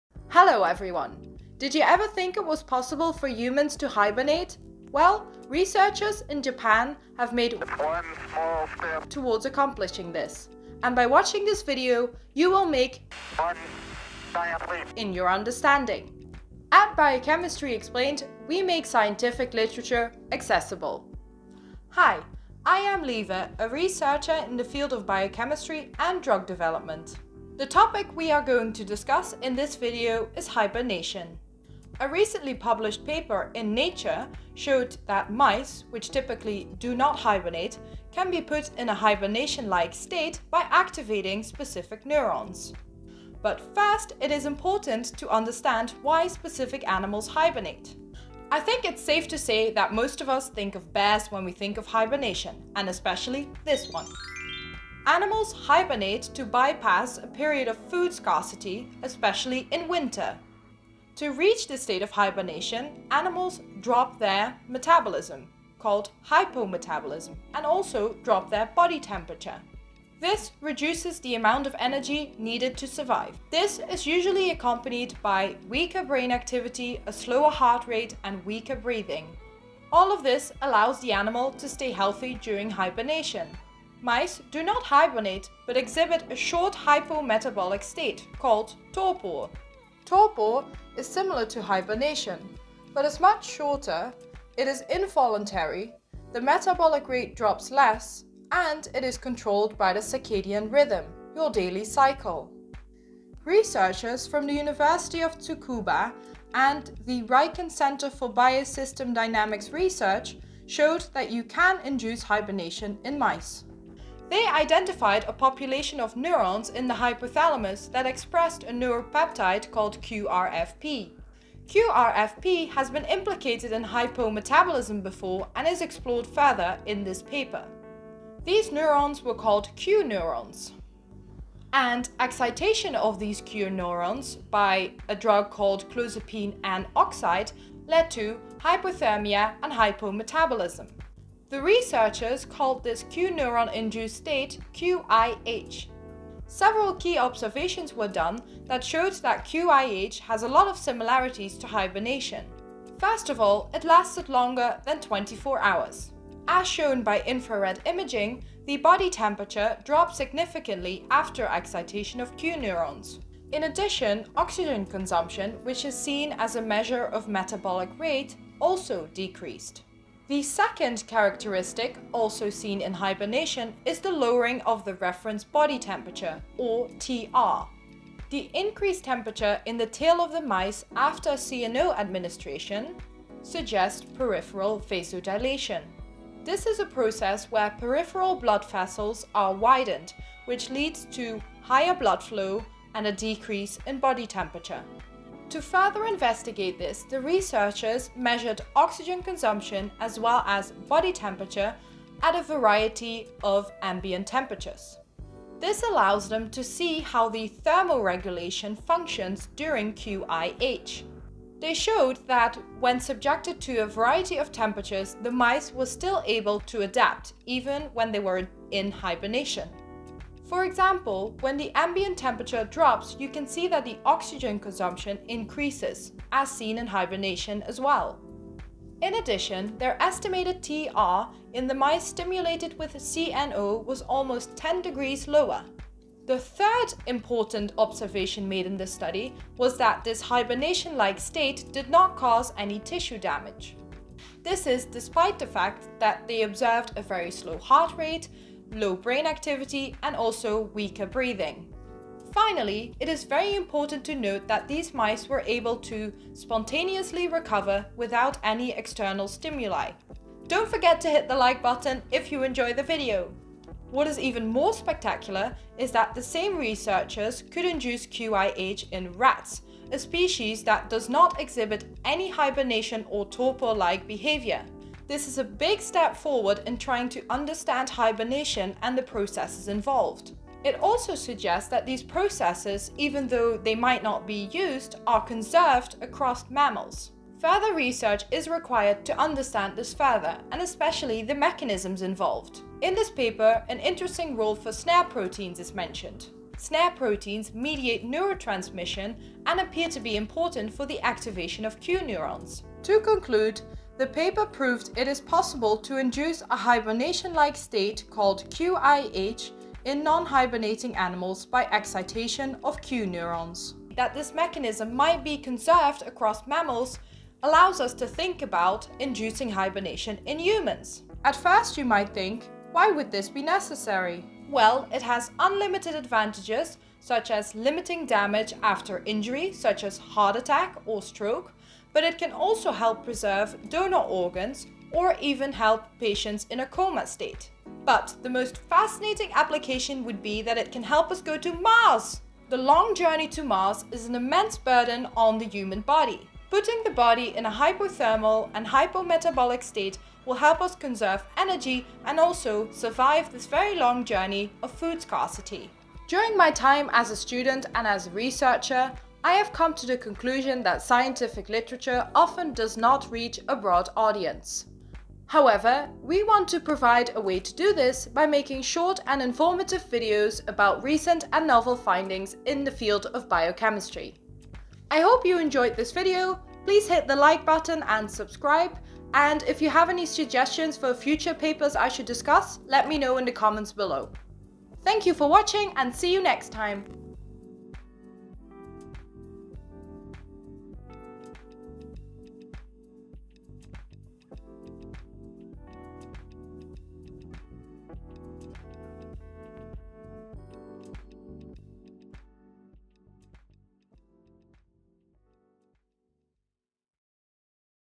How do we get to Mars? -If humans can hibernate, this might do the trick-
Will hibernation help us go to Mars? A recent study published in Nature showed that you can induce a hibernation-like state in mice, a species that does not usually hibernate. The researchers showed that by excitation of certain neurons, mice reached a hypometabolic and hypothermal state highly similar to normal hibernation. This is a really cool study since it allows us to think about inducing hibernation in other mammals, including humans. This might provide a variety of advantages including help us survive the long journey to Mars.

The paper I discussed by Takeshi Sakurai, Genshiro Sunagawa and colleagues can be found here (including figures and videos):

Here are some additional papers related to this topic:
Hypometabolism: